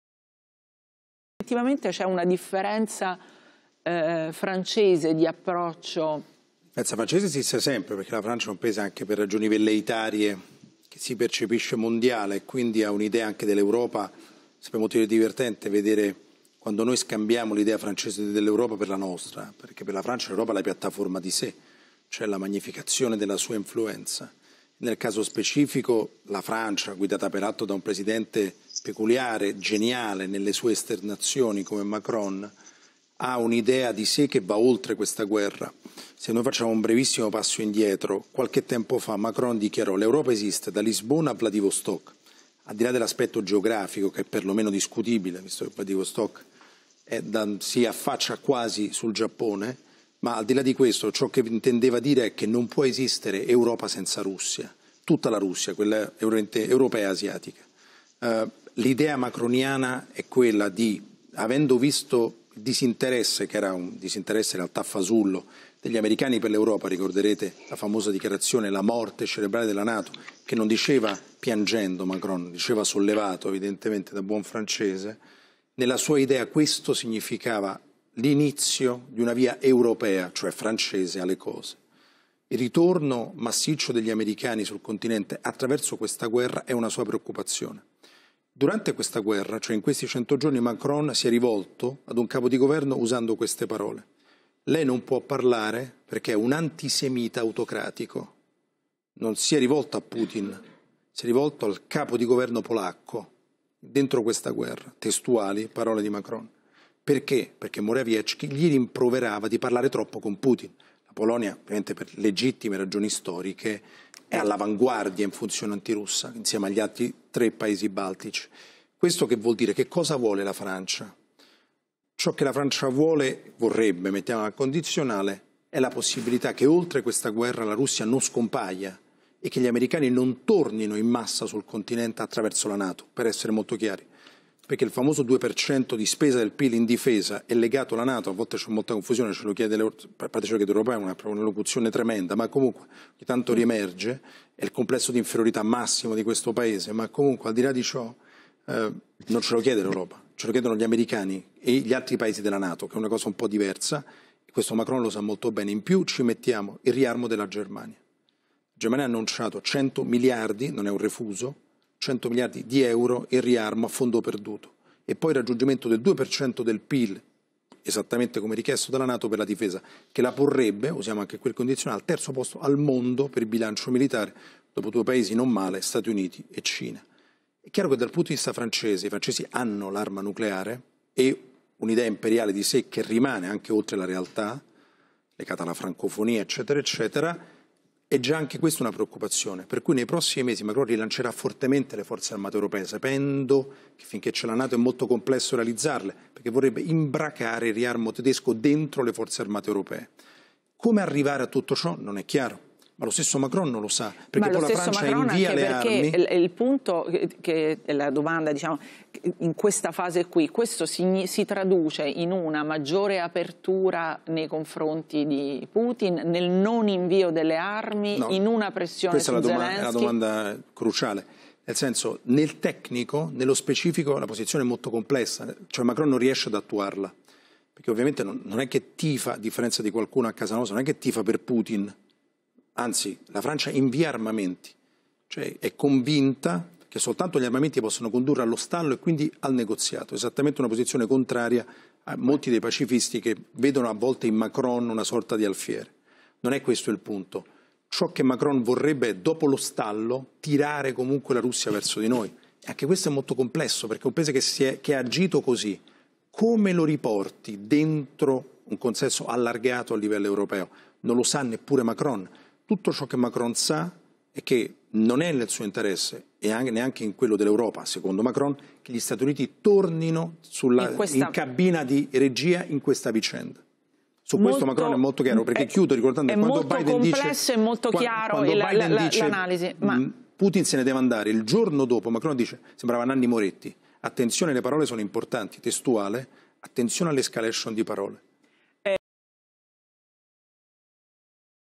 Lo stop alla guerra visto da Macron con gli occhi di Dario Fabbri
Dario Fabbri a la7 sintetizza il punto di vista di Macron su come arrivare allo stop della guerra ( pareggio sul campo tra Russi e Ucraini), recuperare la Russia in una dinamica europea che esaurisce il ruolo della Nato e avvia l'idea di un esercito europeo a guida francese e il ruolo di partner di Germania e Italia